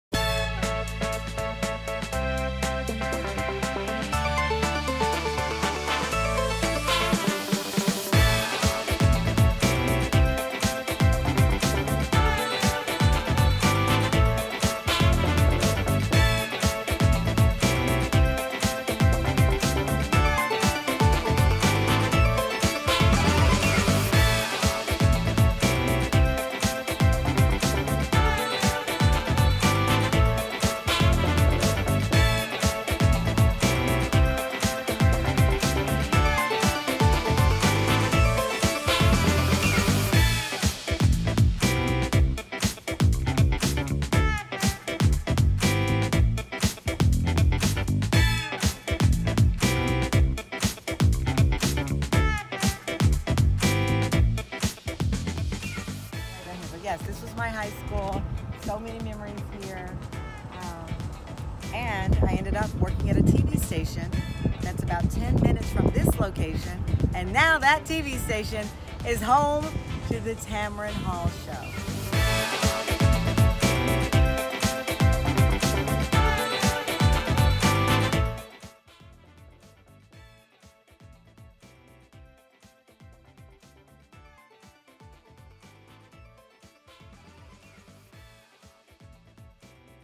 Tamron’s Summer Tour (Part II)
From attending EssenceFest to returning to the Dallas station where her career began, Tamron Hall has had a hot summer!